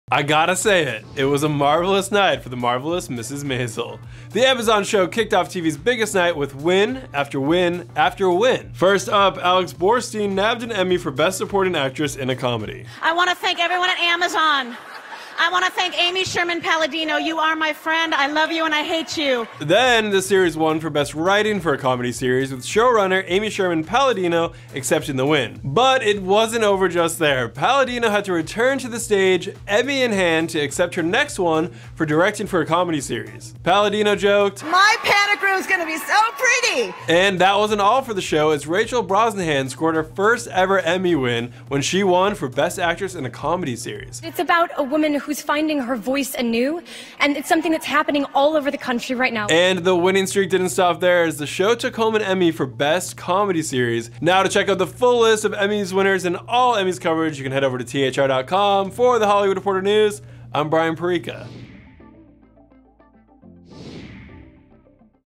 Amazon's 'Marvelous Mrs. Maisel' Sweep the 2018 Emmys | THR News
Rachel Brosnahan also won the Emmy for lead actress in a comedy series for her portrayal of Midge Maisel.